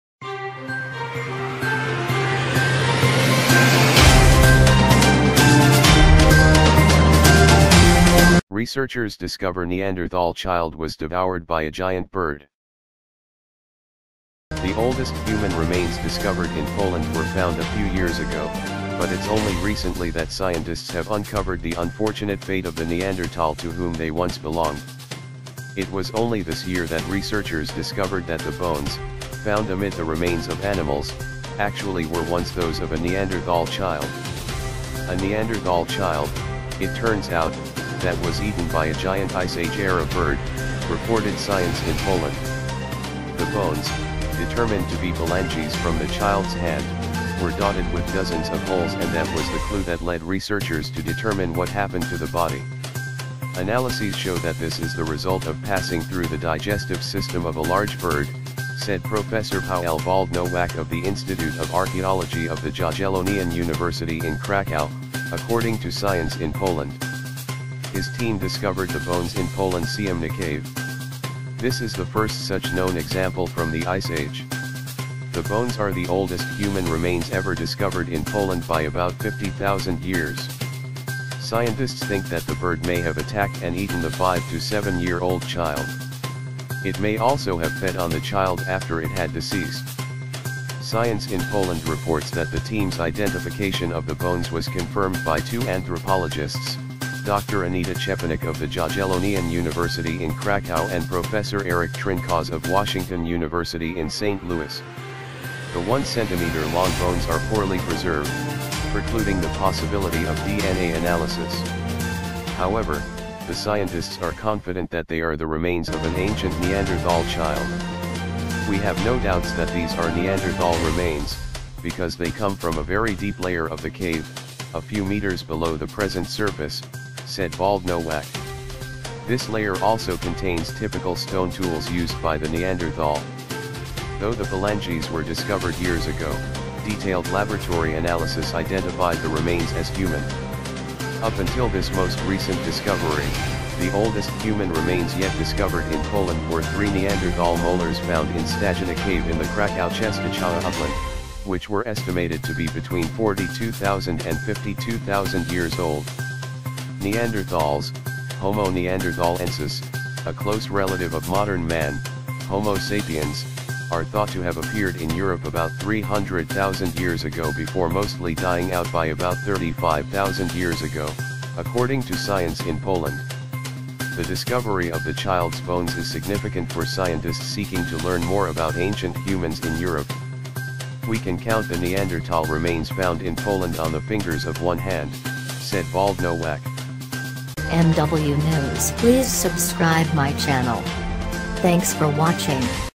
Researchers Discover Neanderthal Child Was Devoured by a Giant Bird | MW NEWS |2018
The oldest human remains discovered in Poland were found a few years ago, but it’s only recently that scientists have uncovered the unfortunate fate of the Neanderthal to whom they once belonged.